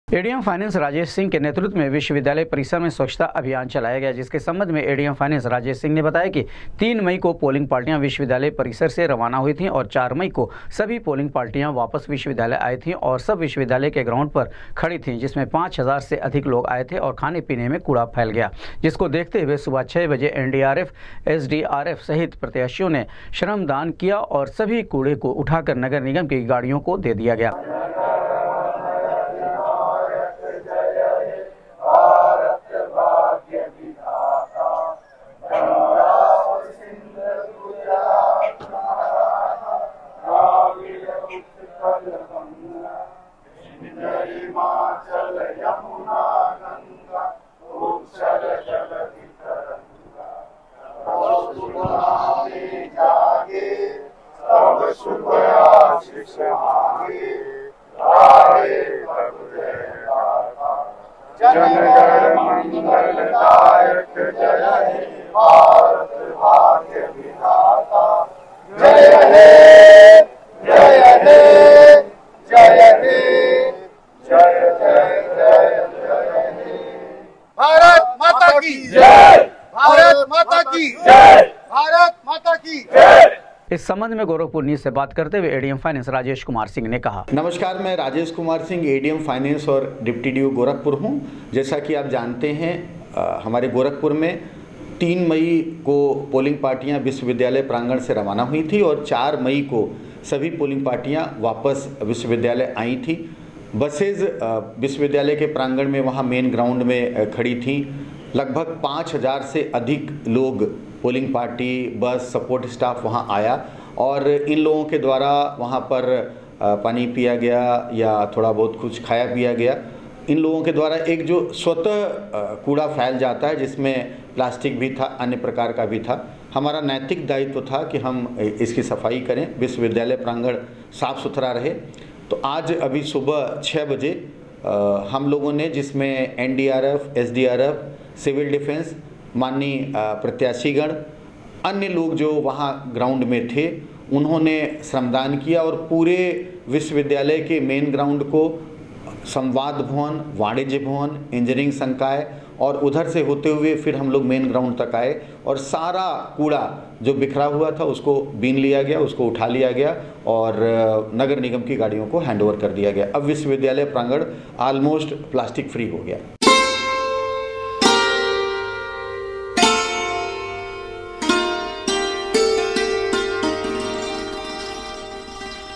एडीएम फाइनेंस राजेश सिंह के नेतृत्व में चलाया गया स्वच्छता अभियान,विश्विद्यालय परिसर में पड़े
एडीएम फाइनेंस राजेश सिंह के नेतृत्व में चलाया गया स्वच्छता अभियान,विश्विद्यालय परिसर में पड़े कूड़े को इकट्ठा करके नगर निगम की गाड़ियों को किया गया सुर्पुद,दिया गया स्वच्छता का संदेश